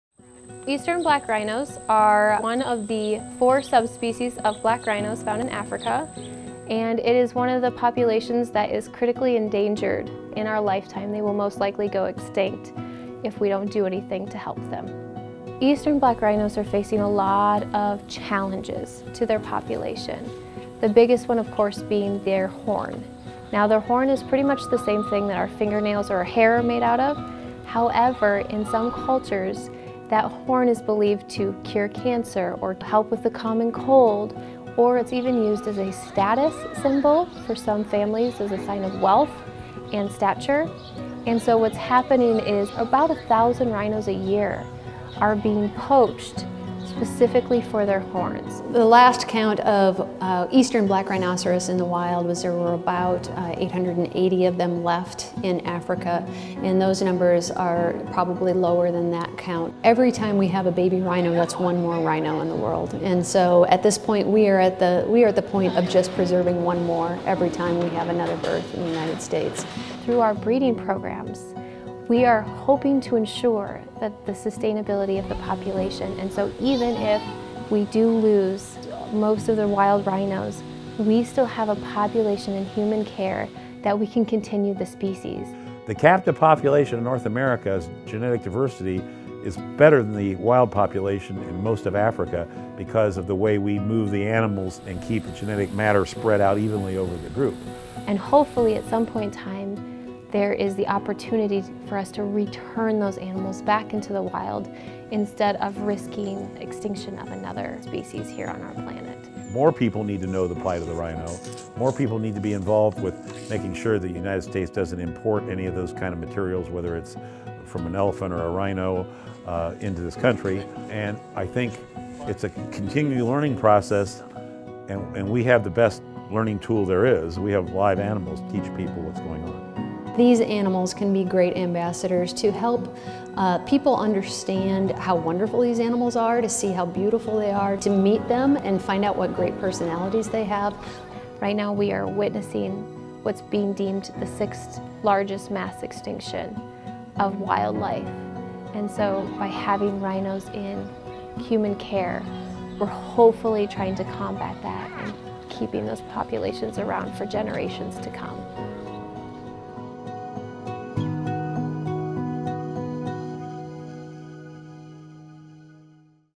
Conservation of Black Rhinos
Black rhinos are facing many threats that have lead to them being critically endangered, but Blank Park Zoo is working to save the species.